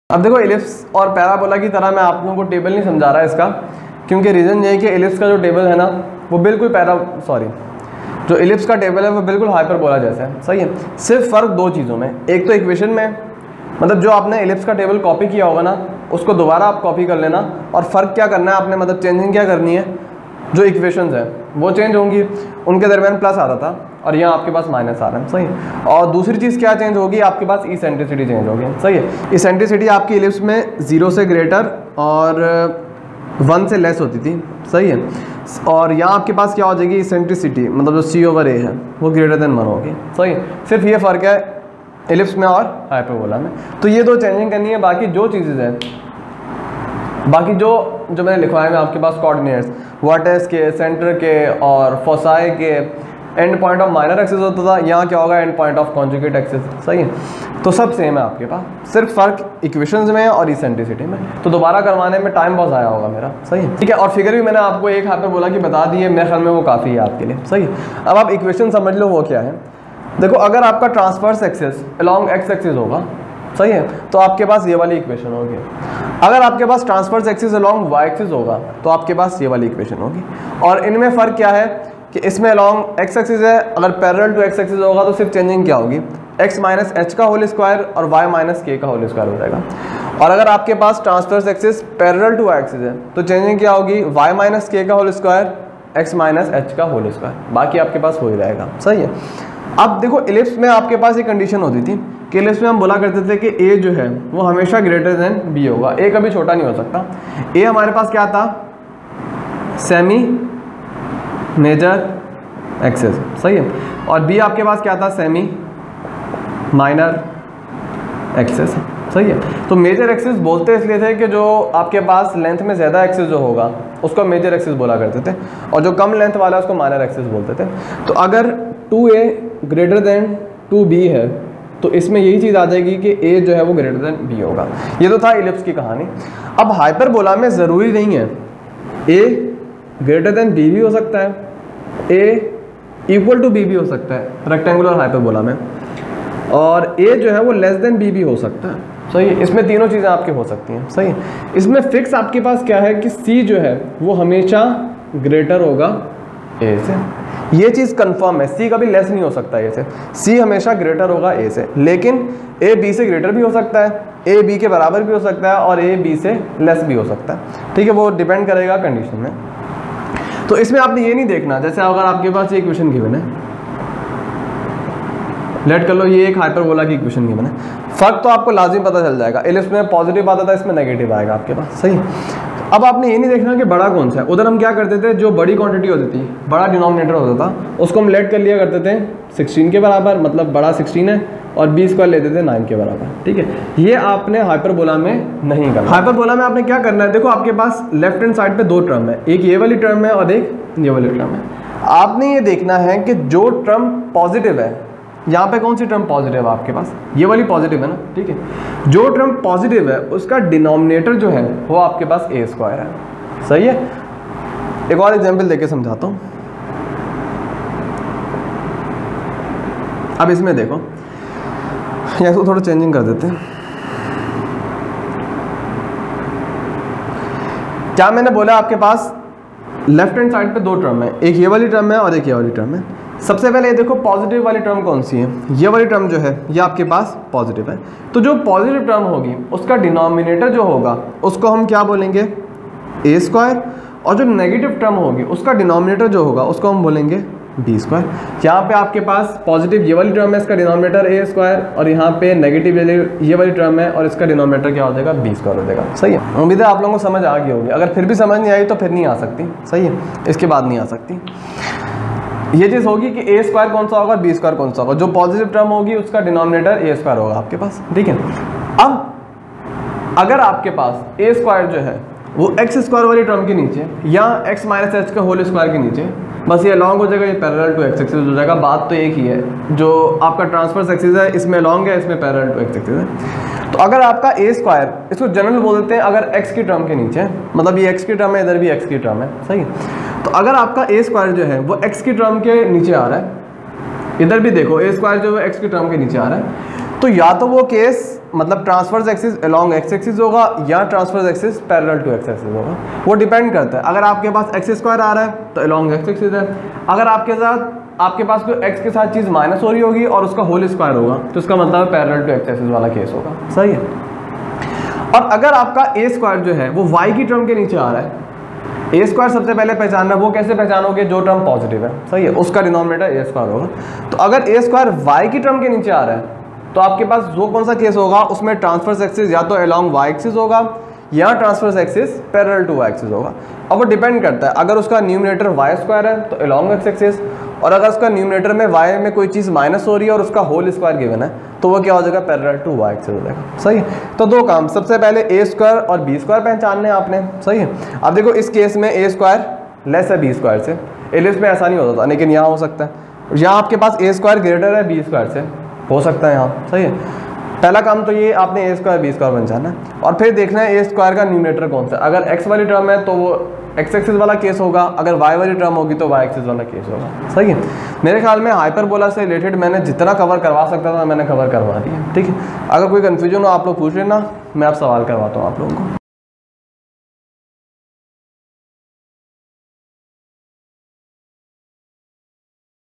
Class 12 Maths | Lecture 150 | Chapter 8 | Introduction to Hyperbola | Asymptote-Eccentricity | Axes
In this video, you will learn:
What is the definition of Hyperbola?
What is an asymptote?
What are the transverse and conjugate axes of Hyperbola?
What is the Eccentricity of Hyperbola?
What are the equations of Hyperbola?